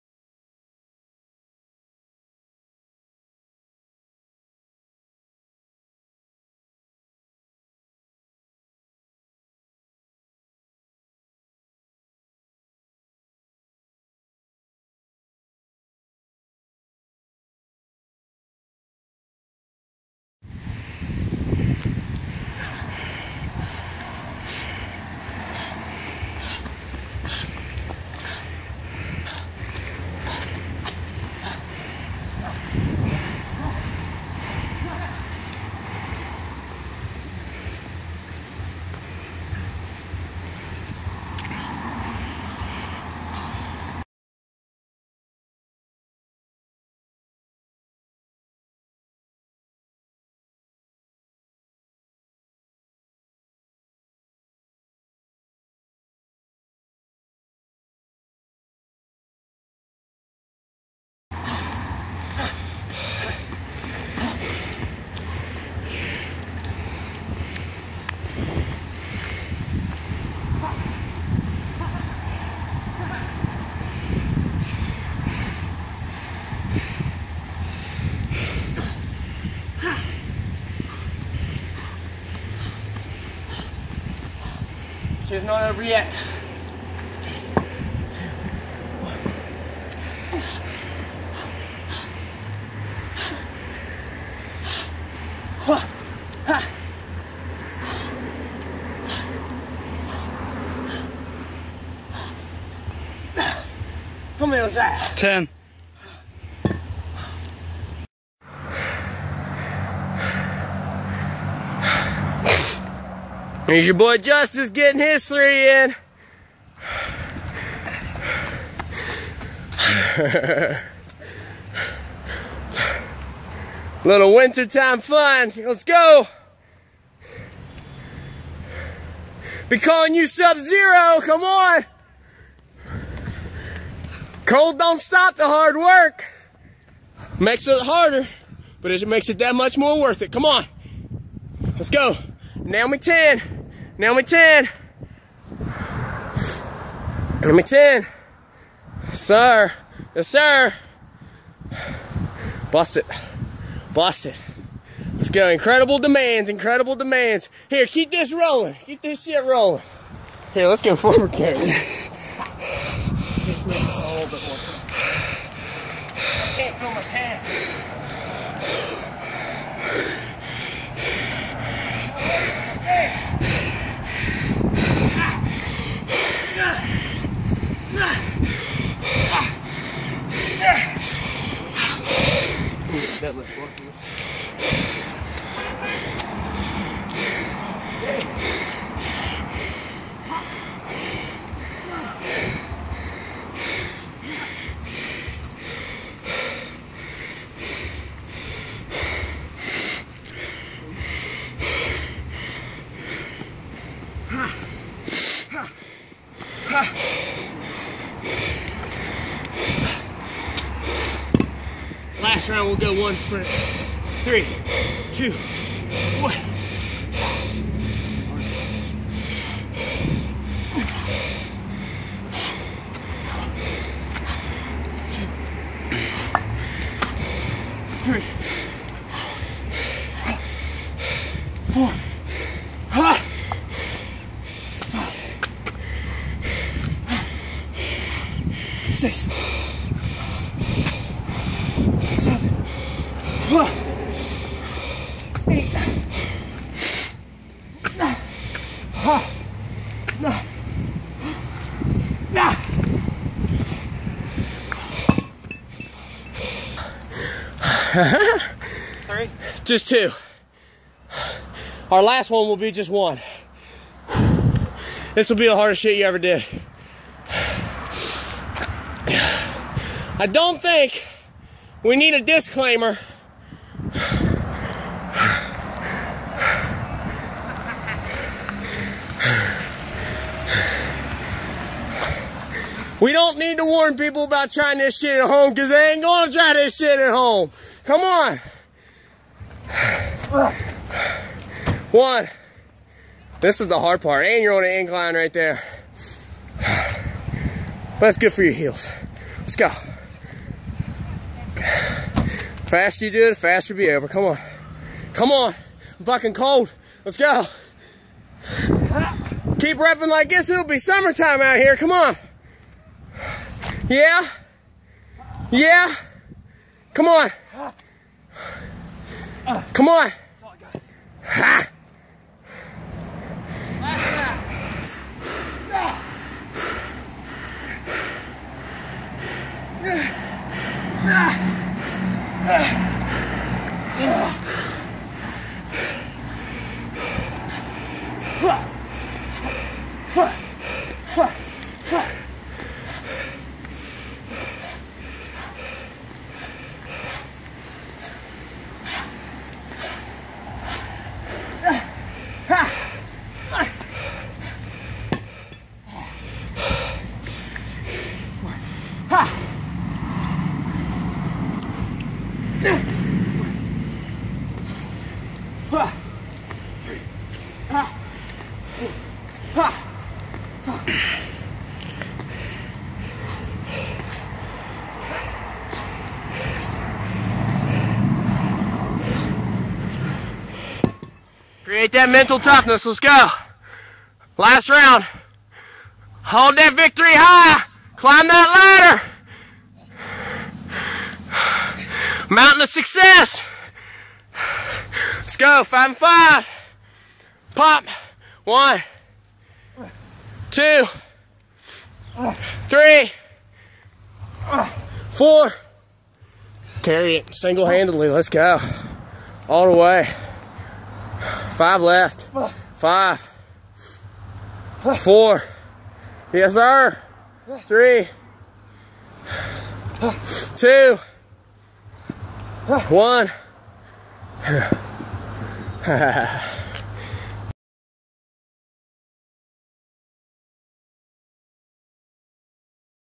Winter Fun with Kettlebells
Here is a little winter time fun from outside of Gorilla Strength. As if pushing sleds and sprinting wasn't enough, I decided to take the fun outside and cool off? Not quite.

I enjoy the cold for mental toughness.

But this workout was completely random for fun.

Mix in a little random to switch things up a bit.